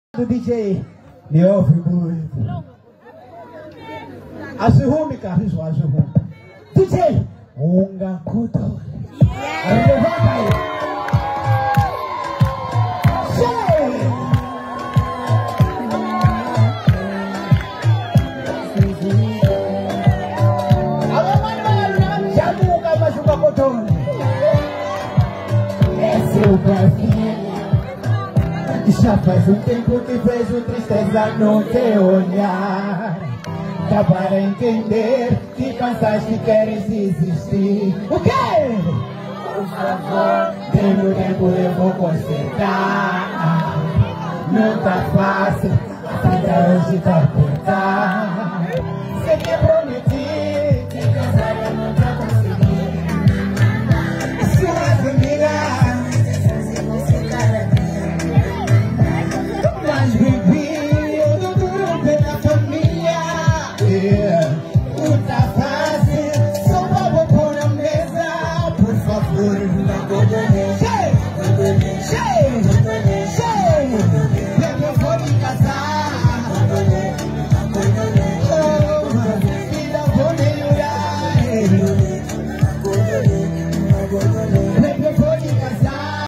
PAI DE AMOR FEZ UM SHOW ESPETACULAR NO DIA 7 DE ABRIL ALUSIVO DIA DA MULHER MOÇAMBICANA
O pai de Kizomba "XAPADA" que sonha fazer featuring com Twenty Fingers" faz um show espetacular que não deixo seguros os líderes, no dia 07 de Abril alusivo dia da mulher Moçambicana.